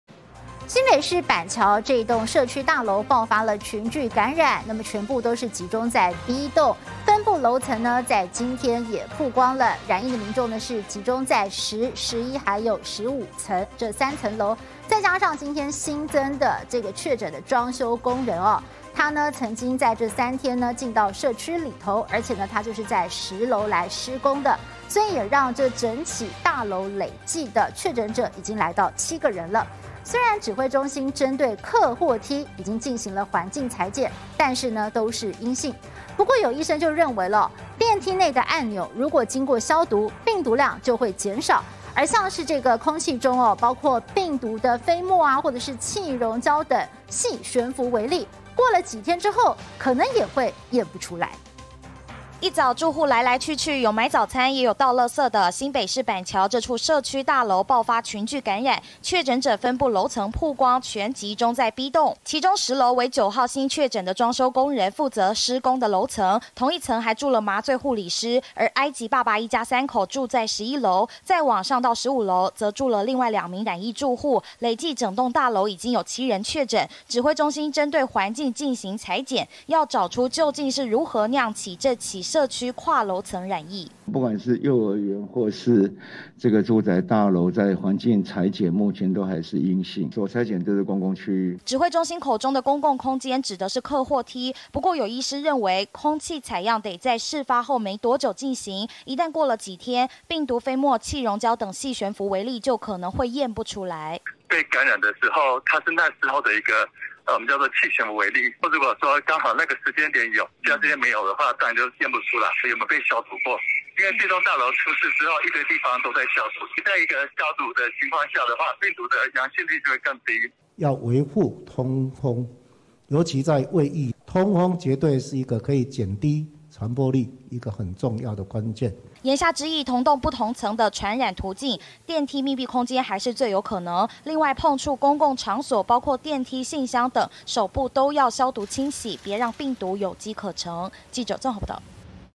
針對客貨梯空氣採樣! 醫:過多天恐驗不出來
新北市板橋某社區大樓，爆發群聚感染，全集中在B棟，分布樓層今天(9)也曝光，染疫民眾集中在10、11及15三層樓，9號新確診的裝修工人跟麻醉護理師，在10樓，埃及爸爸一家3口住在11樓，15樓則住了另外2名染疫住戶，累計大樓內，已經有7人確診，雖然指揮中心針對客貨梯，進行環境採檢，都是陰性，但有醫師說，電梯內按鈕，如果經過消毒，病毒量就會減少，病毒飛沫、氣溶膠等細懸浮微粒，過了幾天也可能會驗不出來。